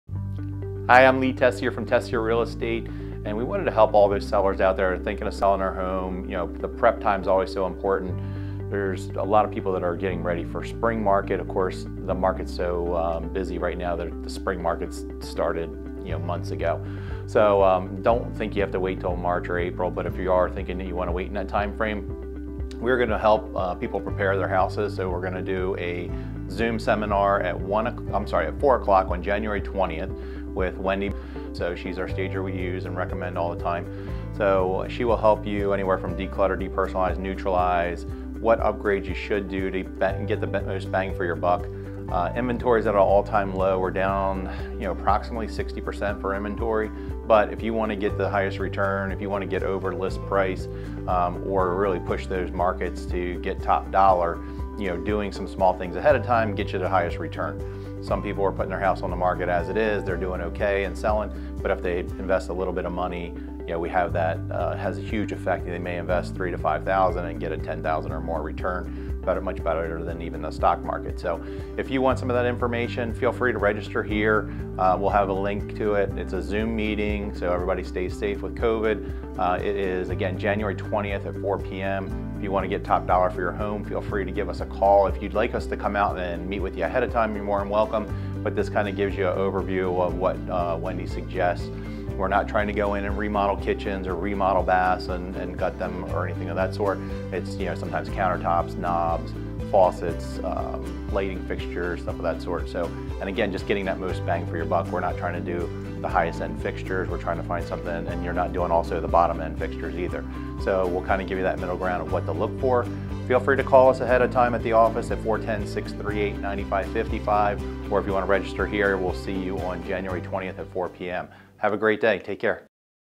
We Are Hosting A Staging Seminar | January 20th, 2021
We are hosting a virtual Zoom Staging Seminar on January 20th, 2021 w/ Wendy Miller of WendMill Designs.